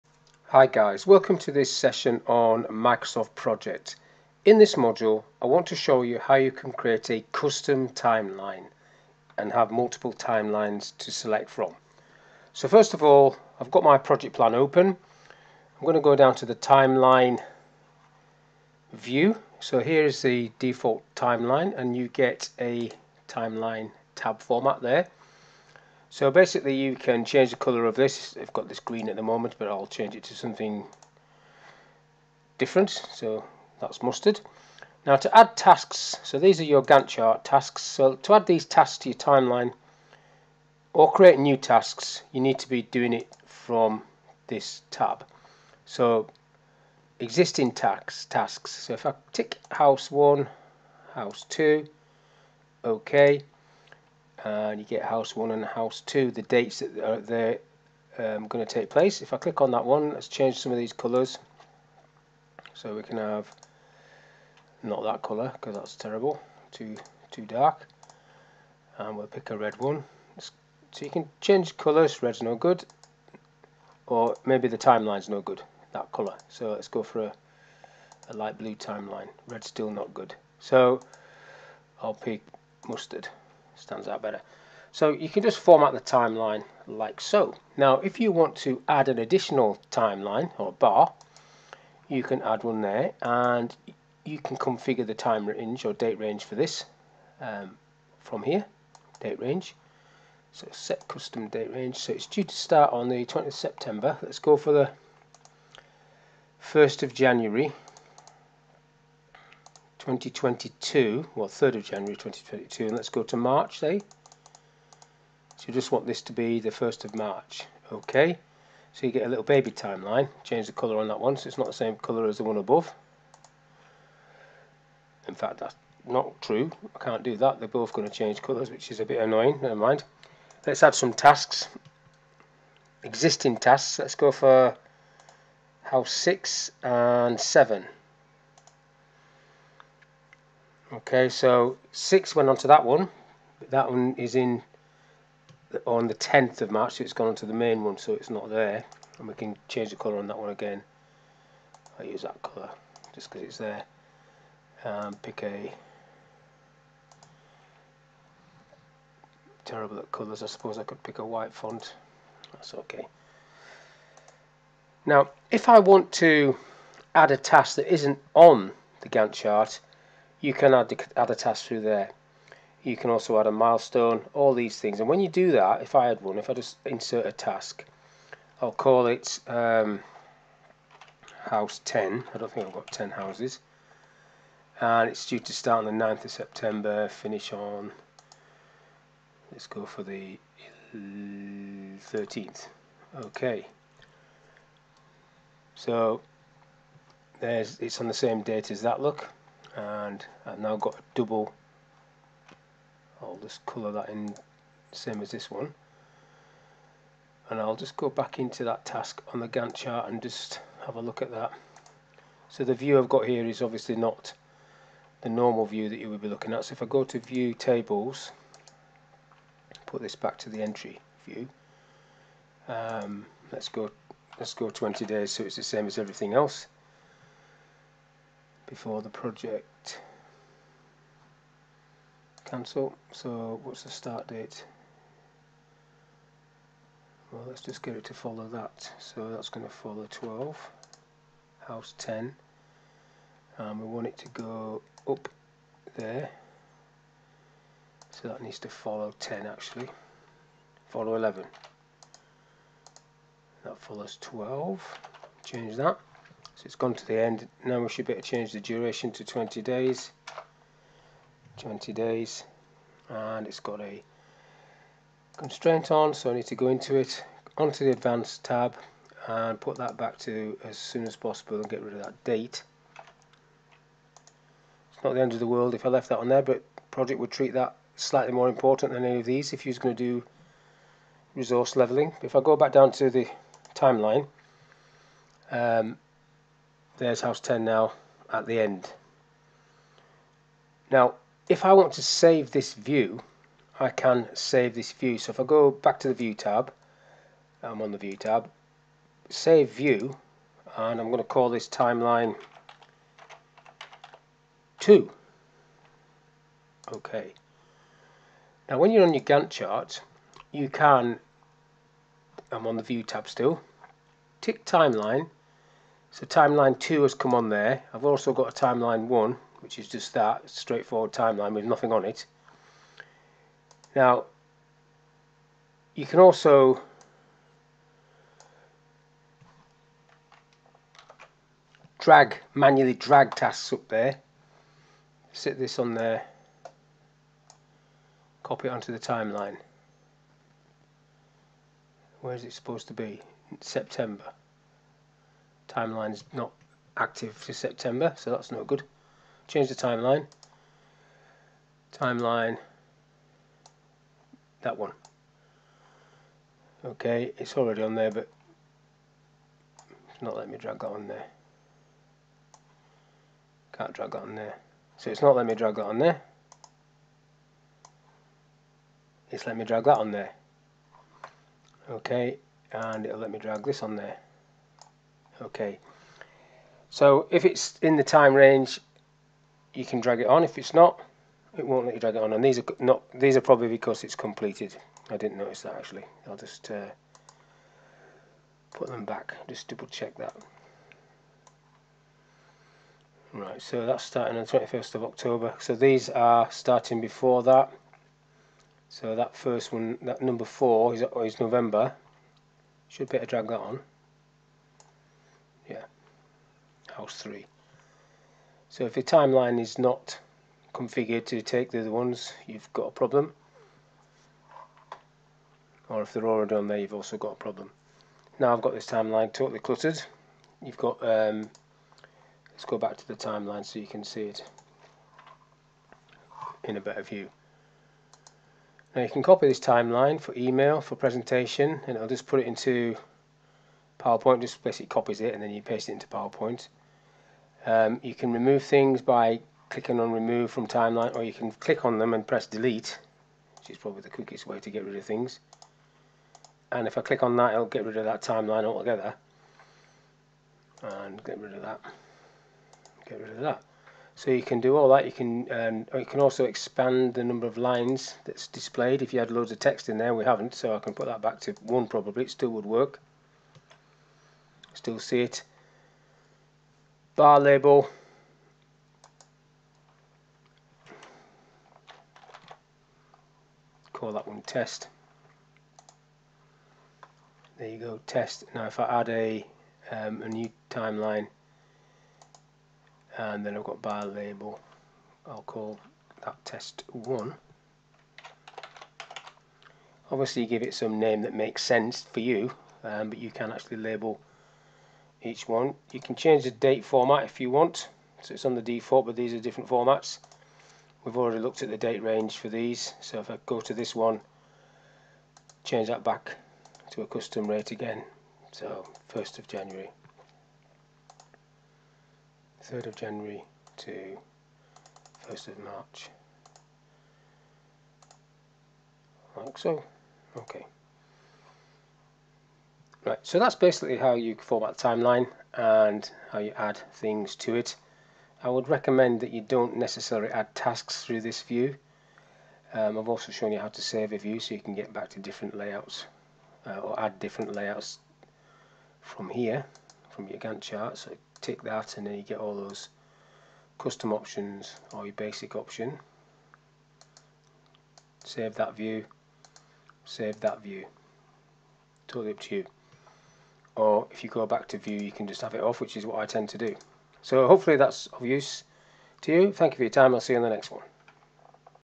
This video explains how to add multiple timelines to Microsoft Project.
The tutor explains how to add multiple timelines in Microsoft Project. Timelines. The tutor covers how to add tasks to a timeline and colour them in. The tutor also mentions how to add a second timeline and change the date range for it. The final part of the video explains how to save your timeline as a view, and then select that view to display above your Gantt chart. This is a Microsoft Project tutorial covering, timelines, timeline views, multiple timelines, formatting timelines and changing timeline date ranges.